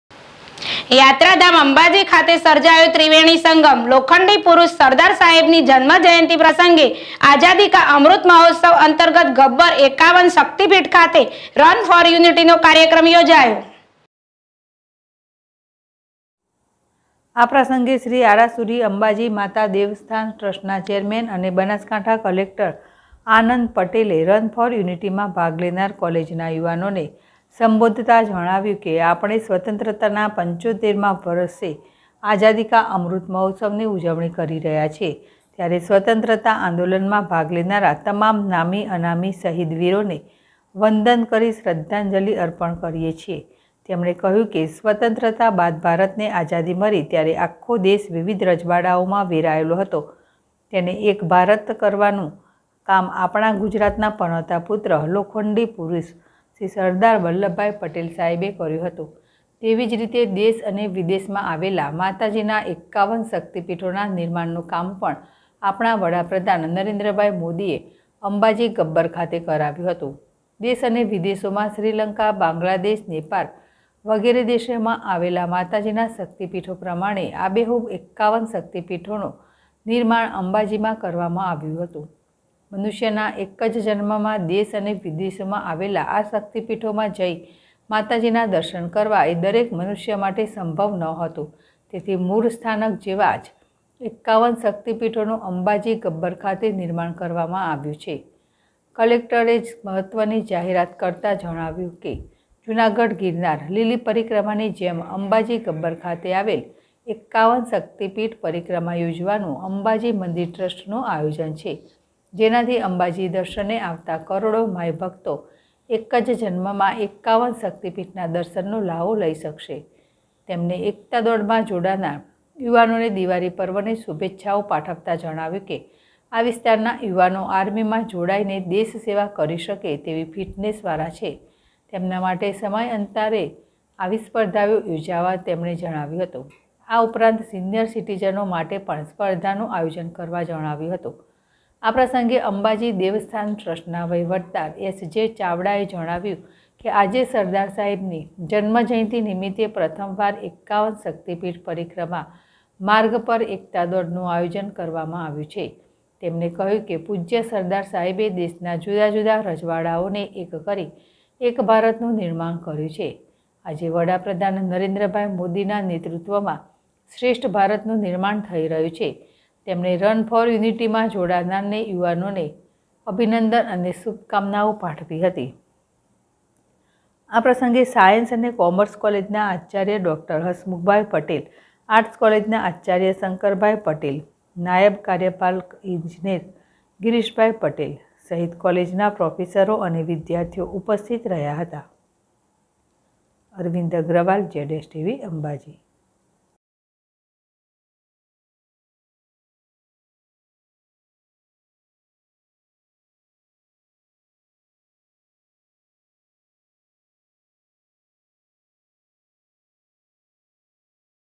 દેશની આઝાદીને ૭૫ વર્ષ પૂરા થવાના અવસરે સમગ્ર દેશમાં આઝાદી કા અમૃત મહોત્સવની ભવ્ય ઉજવણી કરવામાં આવી
દેશની આઝાદીને ૭૫ વર્ષ પૂરા થવાના અવસરે સમગ્ર દેશમાં આઝાદી કા અમૃત મહોત્સવની ભવ્ય ઉજવણી કરવામાં આવી//ZSTVNEWS/